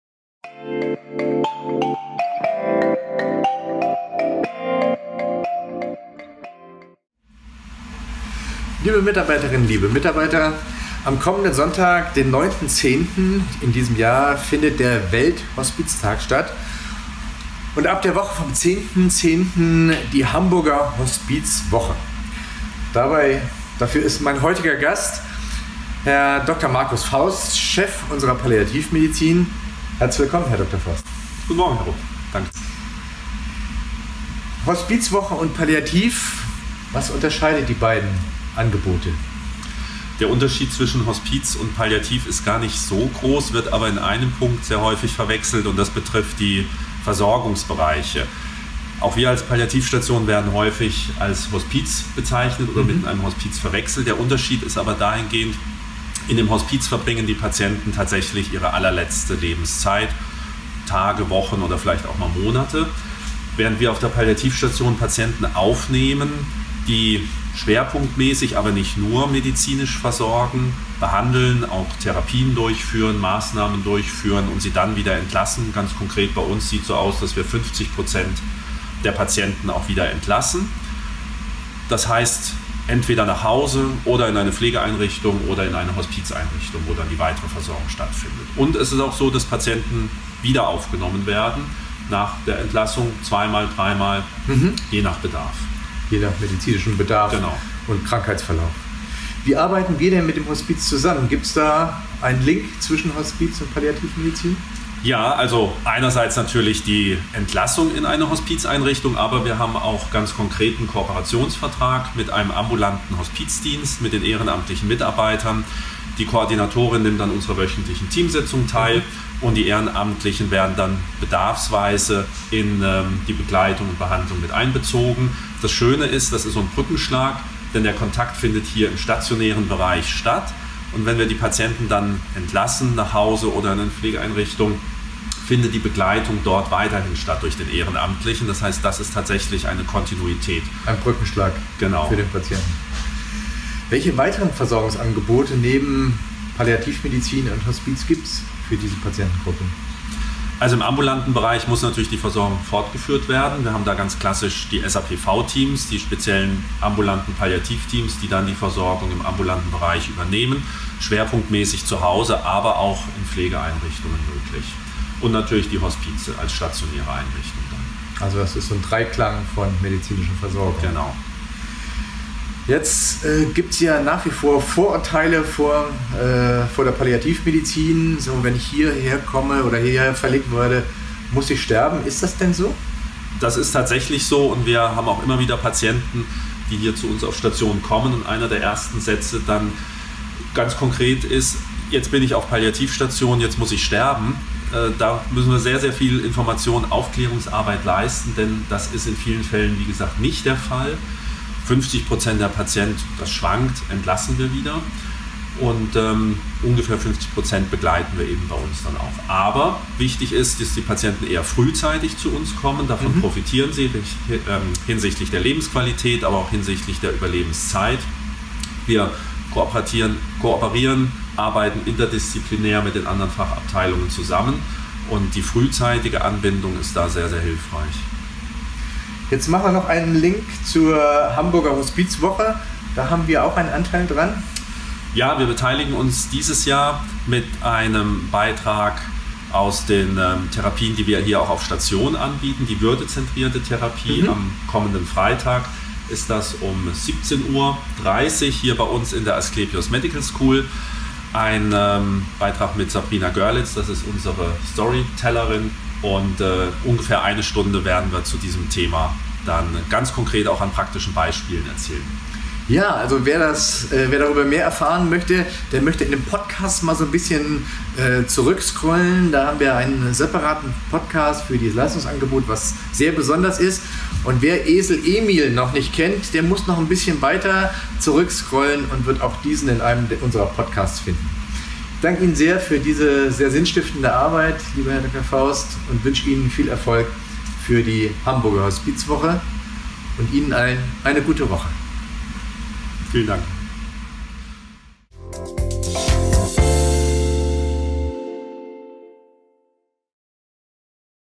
ST. GEORG TALK(T): Hamburger Hospizwoche – Palliativmedizin | Asklepios
Thomas Rupp, Geschäftsführender Direktor der Asklepios Klinik St. Georg, spricht mit Dr. Markus Faust, Chefarzt der Palliativmedizin an der Asklepios Klinik St. Georg, über die Versorgung von Patient:innen mit einer unheilbaren Erkrankung.